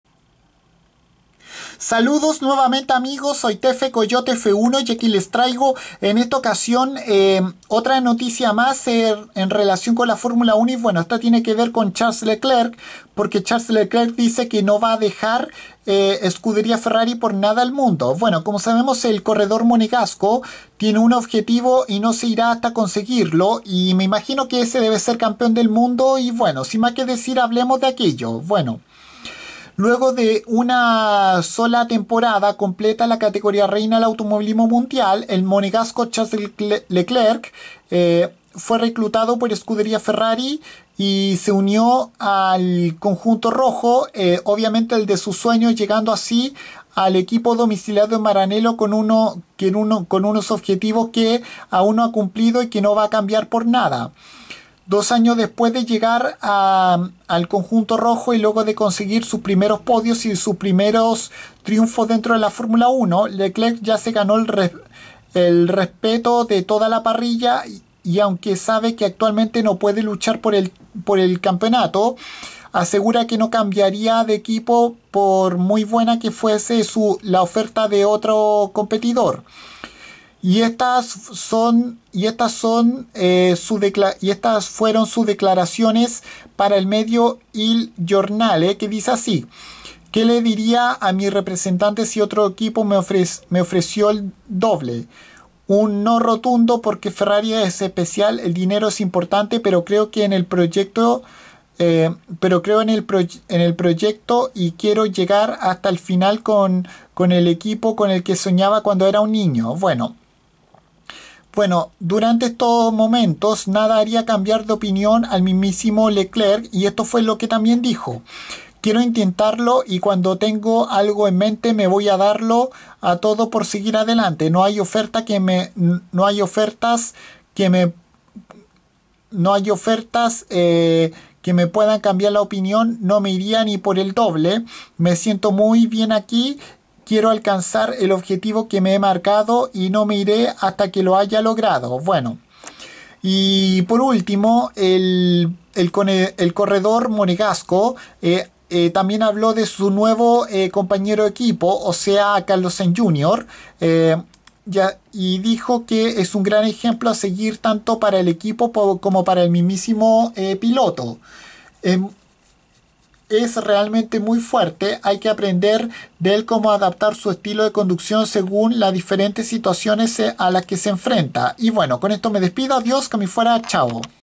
Charles Leclerc dice que no se irá por nada del mundo de Scuderia Ferrari
El corredor monegasco quiere conseguir su sueño de ser Campeón Mundial en la Fórmula 1 con Scuderia Ferrari